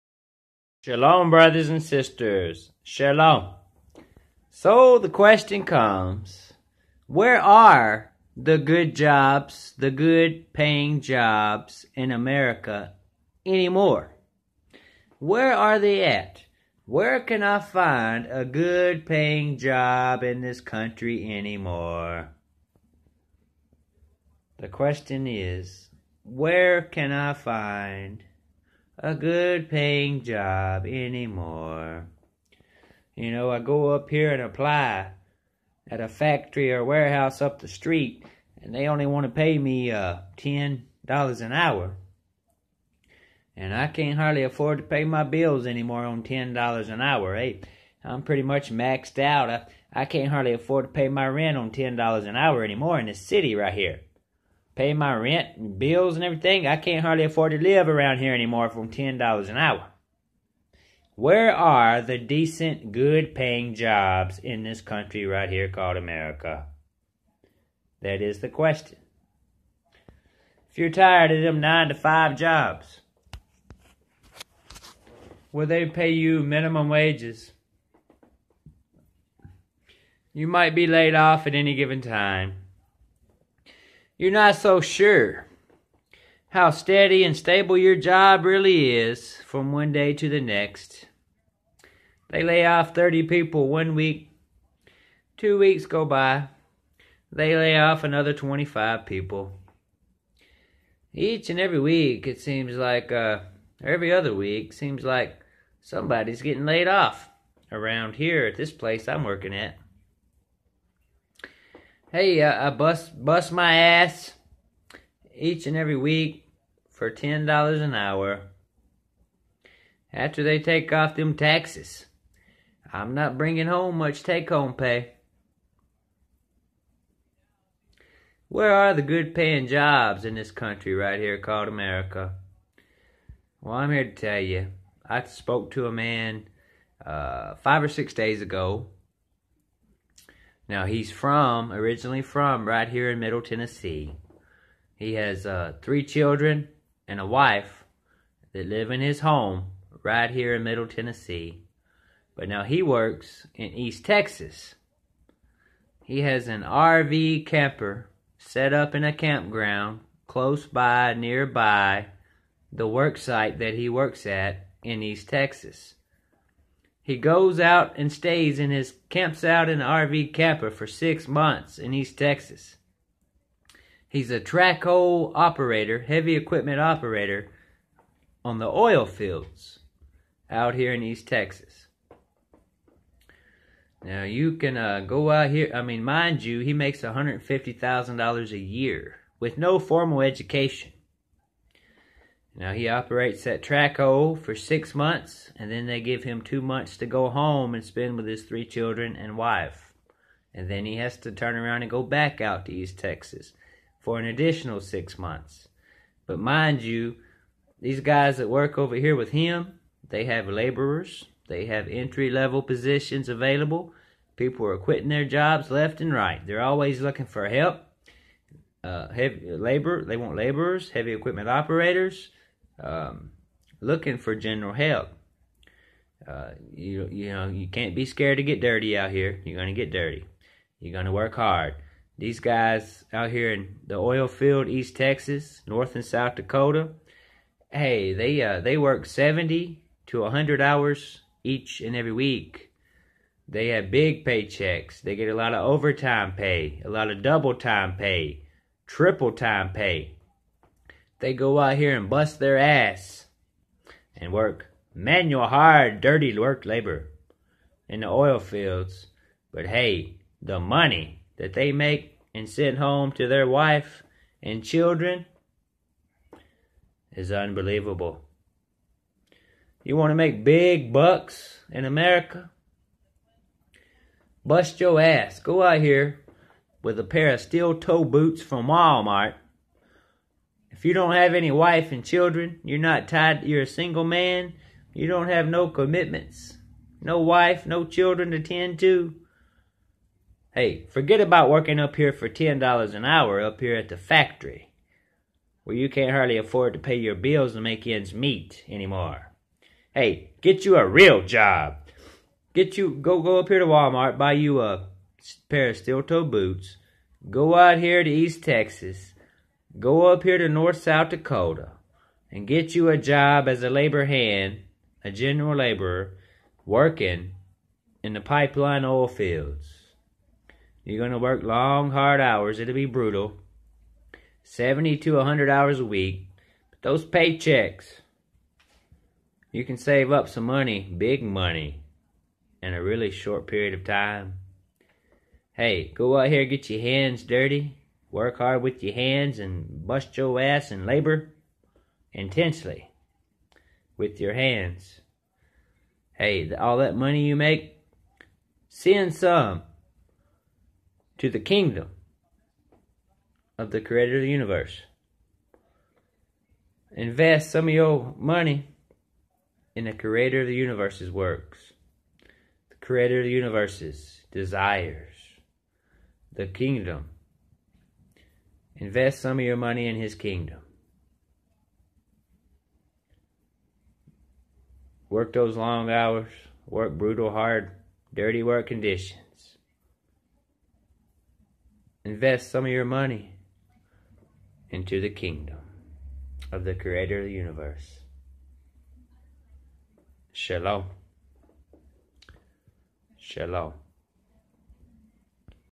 Where Are The Decent Paying Jobs In America?
The good and decent paying jobs in America will NO longer be found locally  up at the local factory or warehouse. Instead you will be forced to travel for very long distances to find good paying jobs.